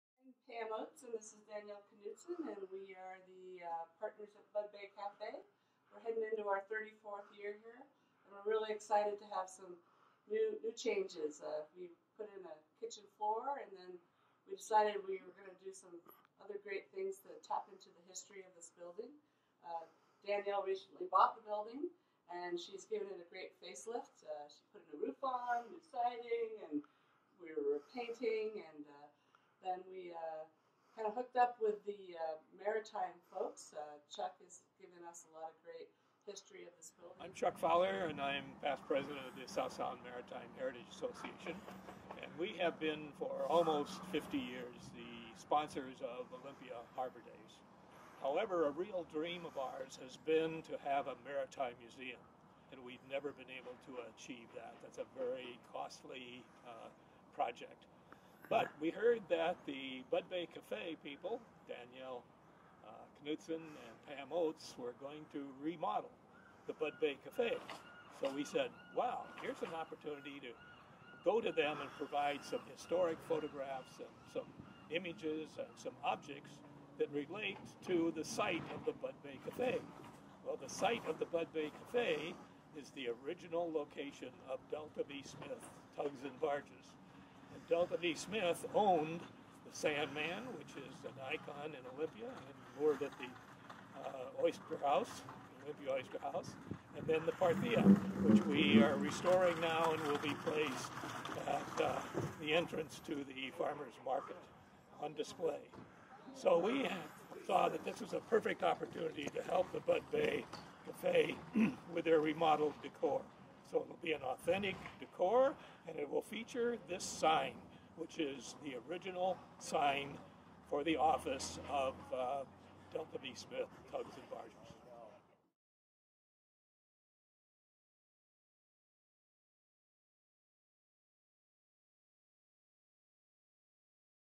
Popular Olympia Waterfront Restaurant Prepares For March 1 Reopening
Business partners Pam Oates and Dannielle Knutson preview the March 1 reopening of the refreshed Budd Bay Cafe. South Sound Maritime Heritage Association past president Chuck Fowler also outlines his organization's new venture with the cafe.